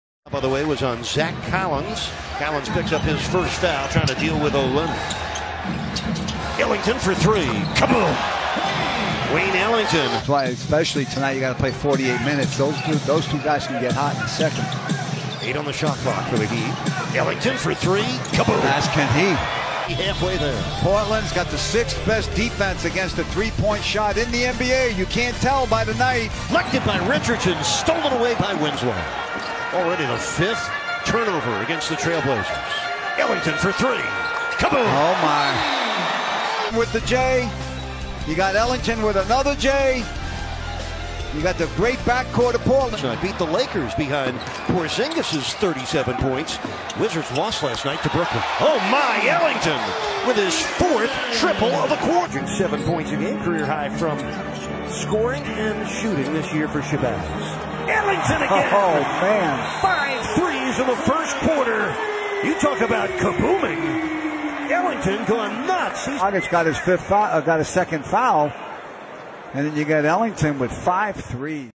Wayne Ellington went 5 5 from three in the first quarter!
I think this guy deserve more minutes.